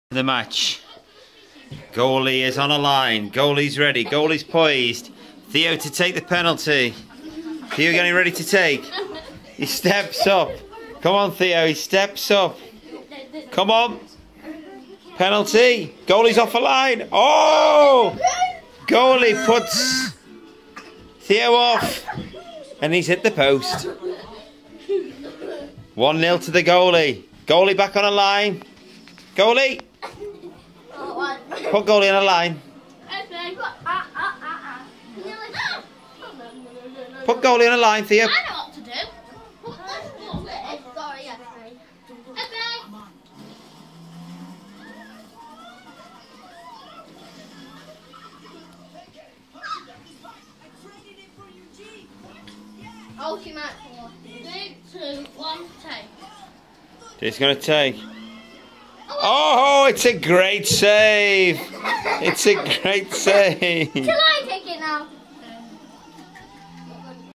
Fun in the lounge at the Pad - Esme aged 6 months
Jacob, Theo and Esme mucked about in the lounge